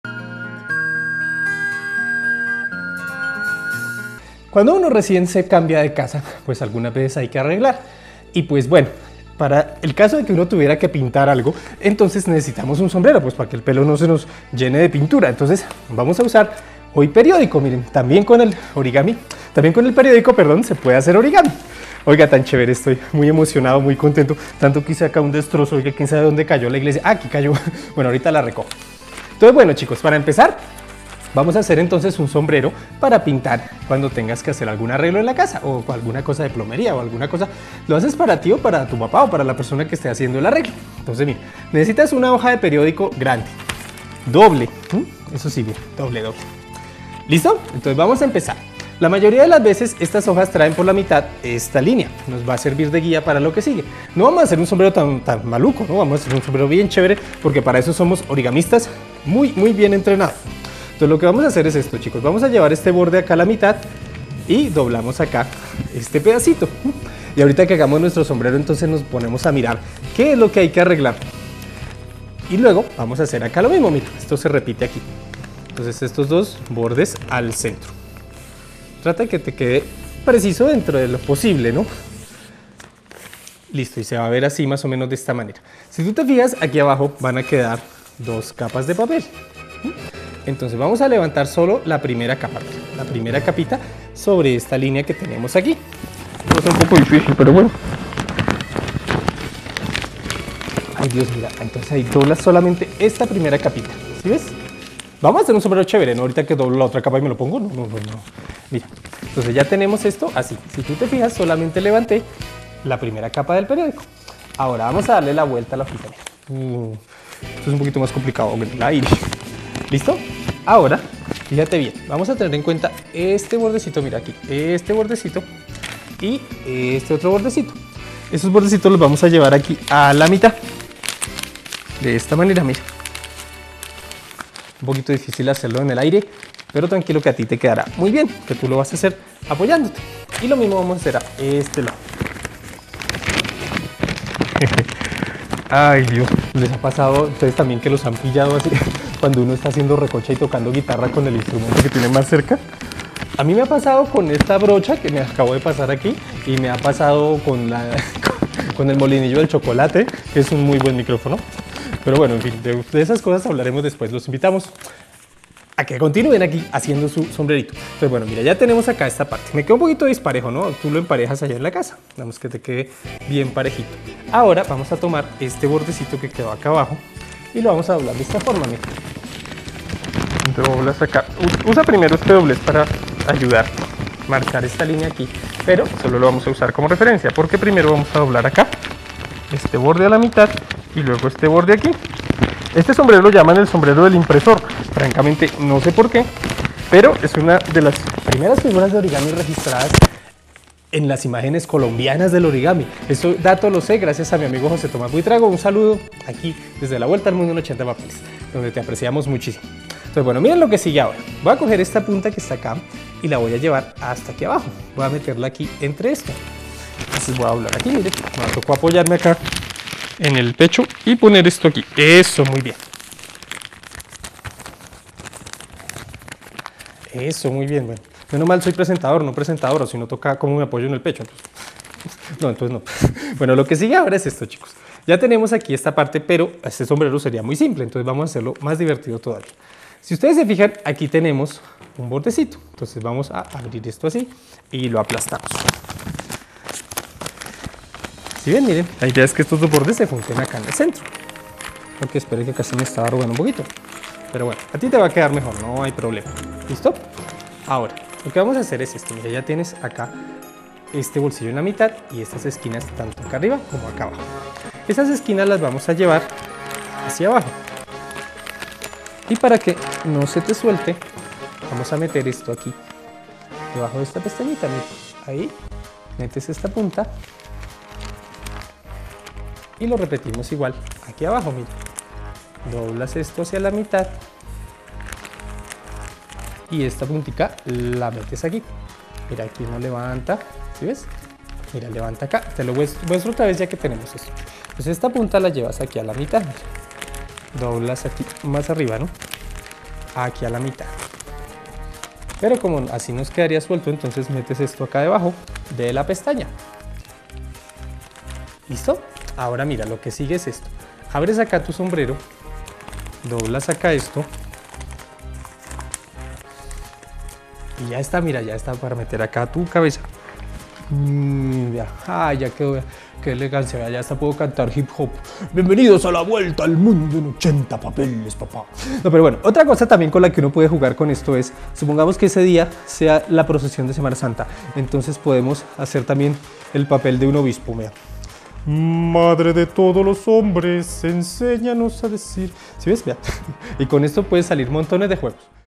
Sombrero en periodico (D.R.A)
Aqui puedes encontrar el famoso "sombrero del impresor" que es muy facil de hacer y puedes usar cuando vayas a darle una mano de pintura a algun lugar de la casa